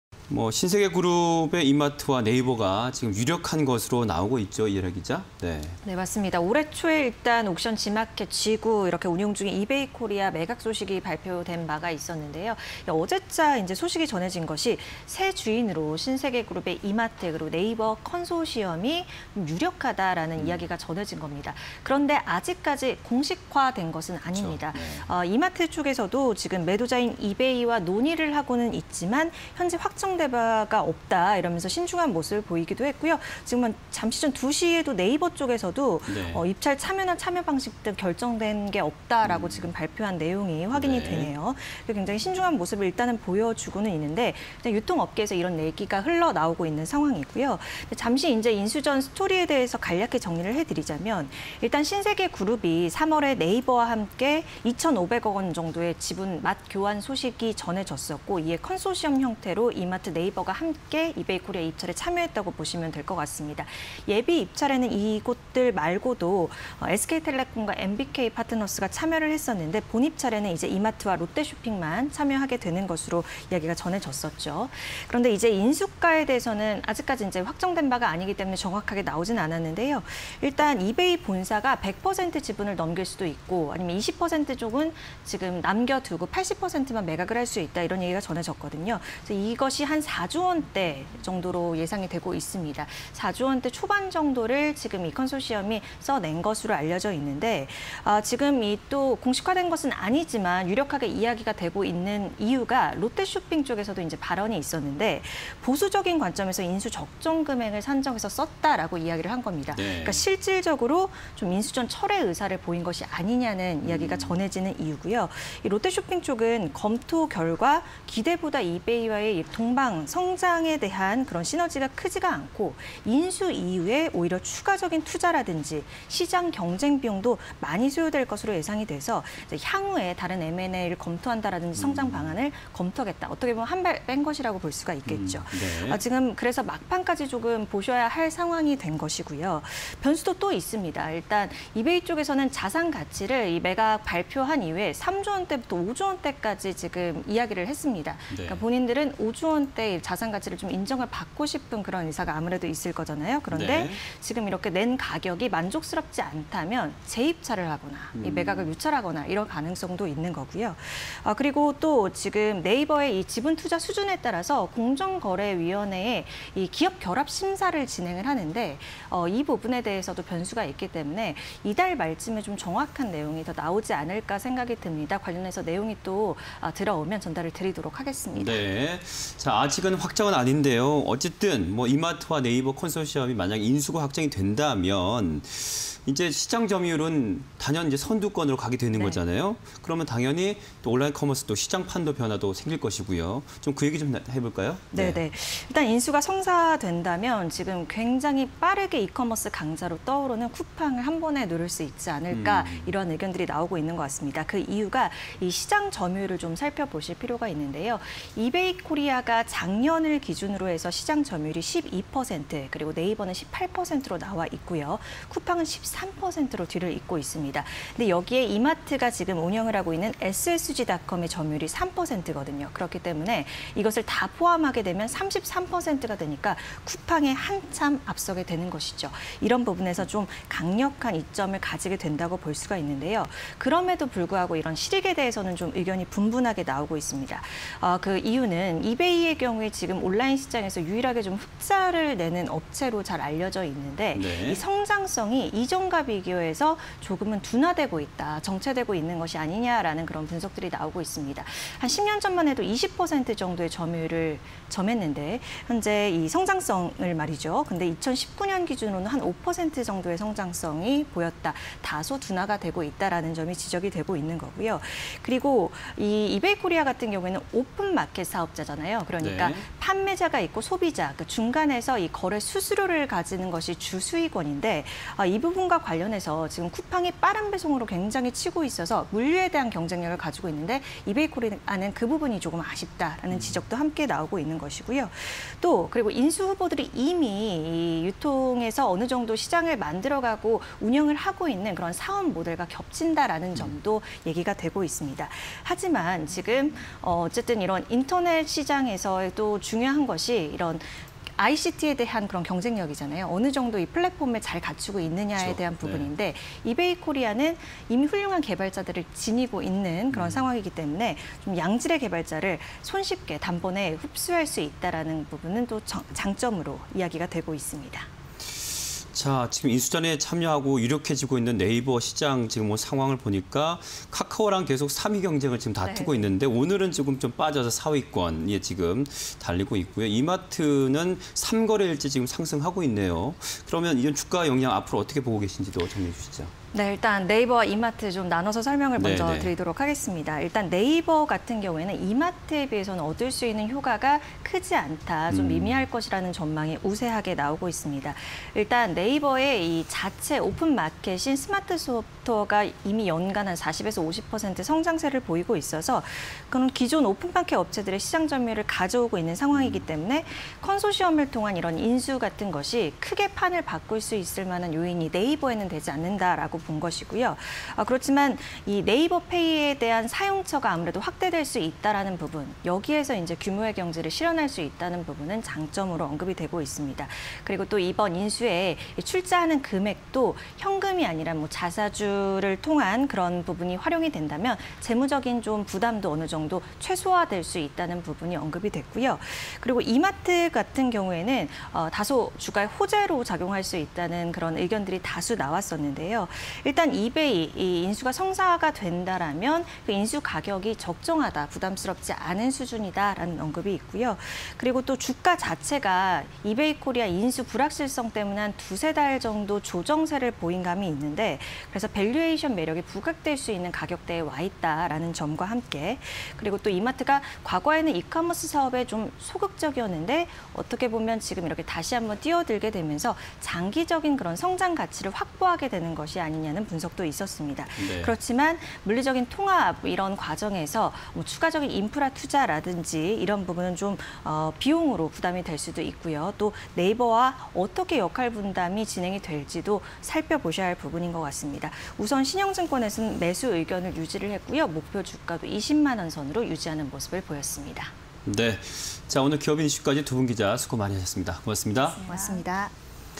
이마트·네이버, 이베이 품는다… 이커머스 지각변동_기업IN이슈 (20210617)
이마트-네이버, 이베이코리아 인수 유력… 인수가 4조 4천억 추정
- 이마트 “논의 중이나 확정된 바 없다”
- 롯데쇼핑 "보수적 금액 산정, M&A 지속 검토"
- 매각 철회· 재입찰 가능성도 제기
- "성장 정체·사업 모델 중복" vs "무형자산 매력적"
- 네이버, 자체 오픈마켓 빠른 성장·페이 사용처 확대 예상
- 이마트, 적정한 인수가·밸류 매력·장기 가치 보유